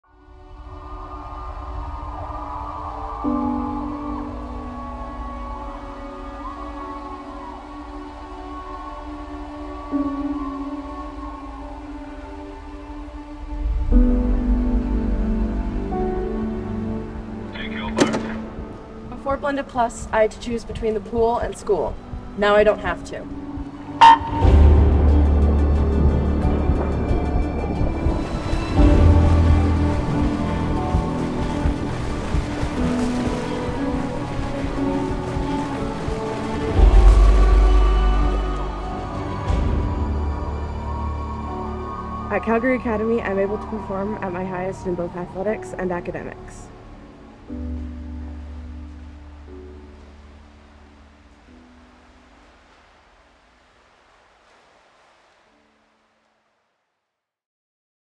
Blended+ | Excelling in Athletics & Academics
For Grade 10 student Karah P., the Blended+ program has been a breath of fresh air, offering a flexible and personalized school experience during her first year. Not only has she thrived academically, but she has also excelled in her chosen sport—competitive swimming.

Blended+ has provided Karah with the perfect platform to pursue her passion for swimming while maintaining a strong academic foundation. The program's emphasis on flexibility and independence has allowed her to create a school schedule that accommodates both her training sessions and competitions. The personalized approach to learning has also enhanced her engagement in the school community due to the many opportunities to be involved in the CA community. Now that she has finished her first year in the program, Karah has found a true sense of belonging at Calgary Academy.

According to Karah, “At Calgary Academy, I’m able to perform at my highest in both athletics and academics.”

Limited spaces are available for Grades 9, 11 and 12 students interested in joining the Blended+ program for the 2023-24 school year.